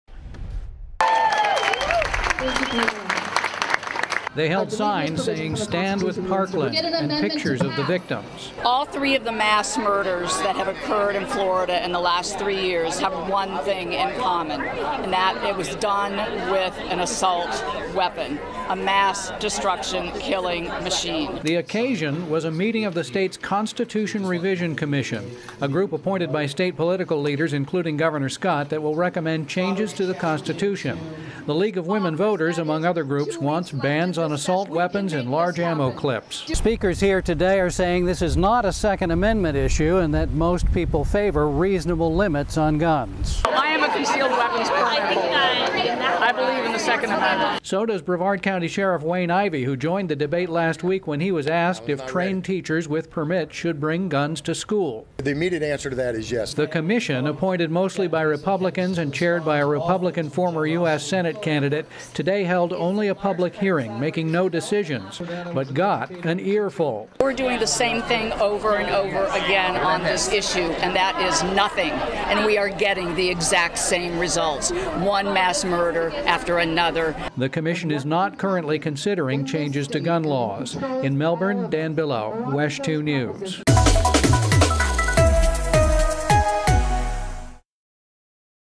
Group calls for ban on assault weapons
Dan Billow reports.